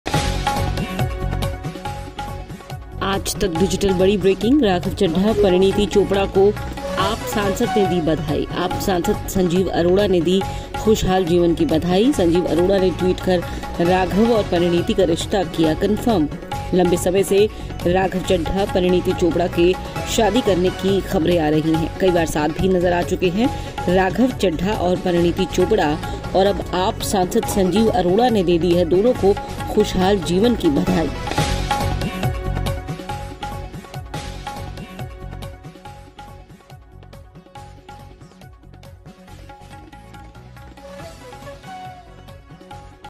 Breaking News: Raghav Chadha-Parineet Chopra को AAP सांसद ने दी Twitter पर दी खुशहाल जीवन की बधाई
बॉलीवुड एक्ट्रेस परिणीति चोपड़ा इन दिनों अपने रिलेशनशिप की खबरों को लेकर सुर्खियों में हैं. परिणीति का नाम आम आदमी पार्टी (AAP) के सांसद राघव चड्ढा संग जोड़ा जा रहा है. खबरें हैं कि दोनों एक दूसरे संग रिलेशनशिप में हैं. परिणीति और राघव चड्ढा की डेटिंग की चर्चाओं के बीच अब AAP सांसद संजीव अरोड़ा ने अपने ट्वीट से दोनों के रिश्ते को कंफर्म कर दिया है.